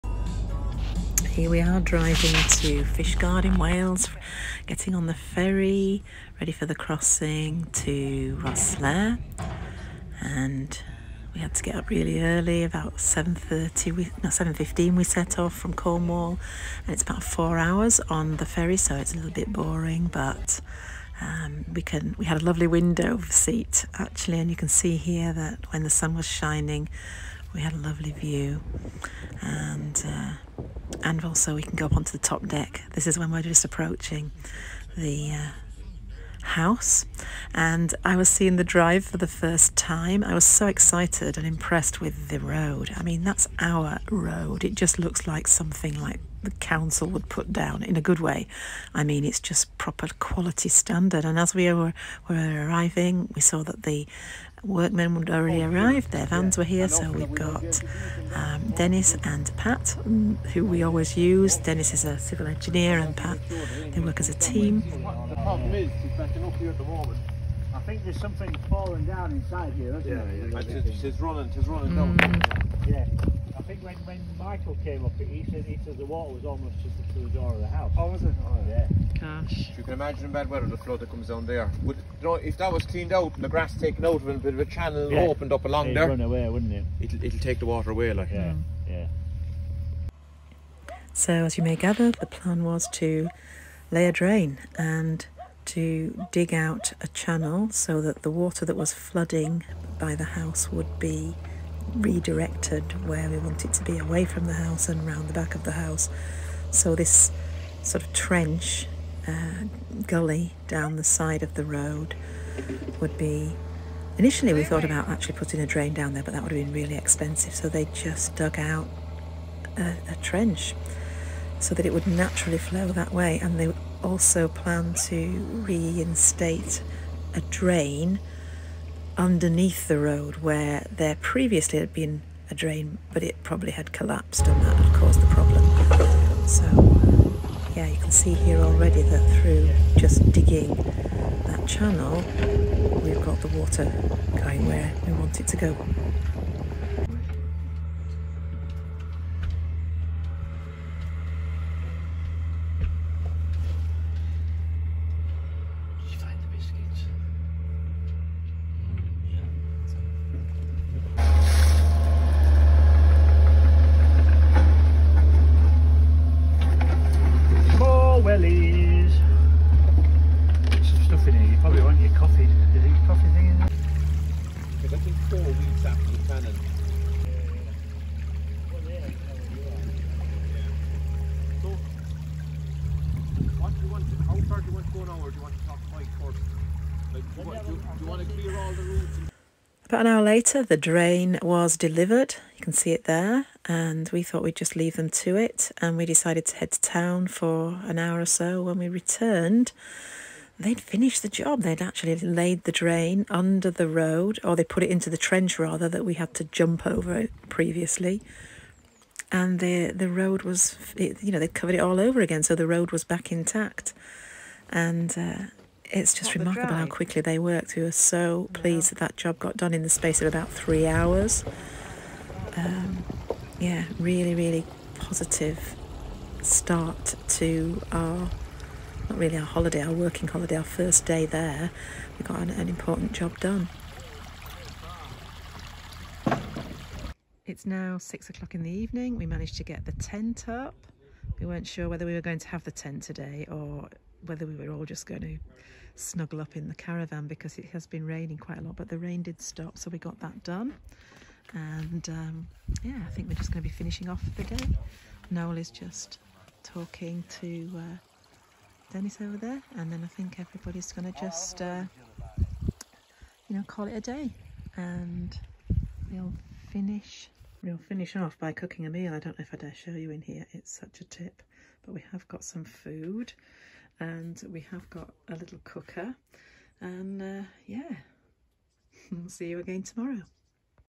Digging a Drain IN 3 HOURS! | EP004
VLOG 4 An underground drain must have collapsed so the ground in front of the house had flooded. It was necessary to get proper drainage sorted as a matter of urgency.